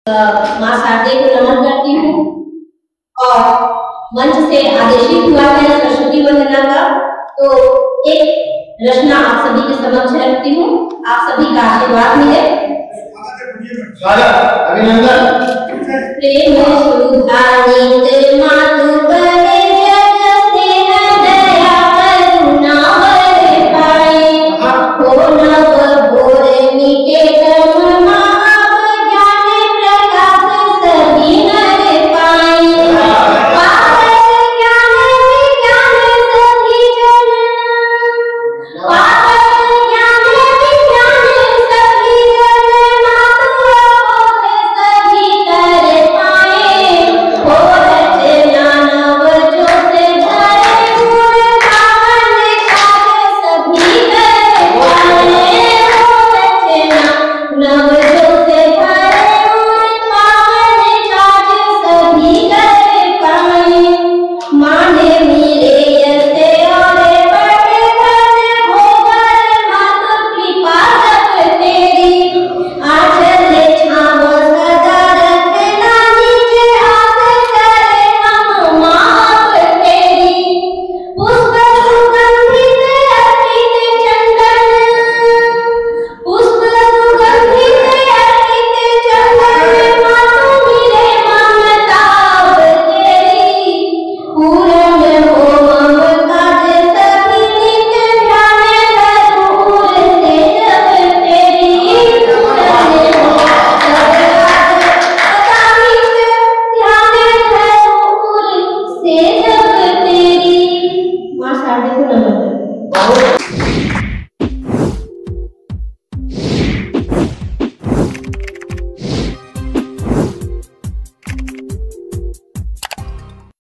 प्रियंका गुप्ता ने की अद्भुत अंदाज में माँ शारदा वंदना
अखिल भारतीय कवि सम्मेलन
रविवार, दि. 07 मार्च 2021, संध्या 6 बजे से रात 10 बजे तक
स्थल : डॉ.बाबासाहेब आंबेडकर सभागृह, कुंभारे नगर, गोंदिया

आमंत्रित कविगण
श्री दिनेश देहाती, बालाघाट (मध्यप्रदेश) - मंच संचालन
श्री रामबृक्ष गुप्ता ‘अहसास’, नागपुर (महाराष्ट्र)
श्री बंशीधर मिश्रा, बिलाईगढ़ (छत्तीसगढ़)
श्री देवेंद्र परिहार, मुंगोली (मध्यप्रदेश)
श्री असीम आमगाँवी, आमगाँव (महाराष्ट्र)
श्री साहिल कुमार, नागपुर (महाराष्ट्र)
श्री हेमंत मोहारे, आमगाँव (महाराष्ट्र) - संयोजक

विनीत
श्री अरुण गजभिये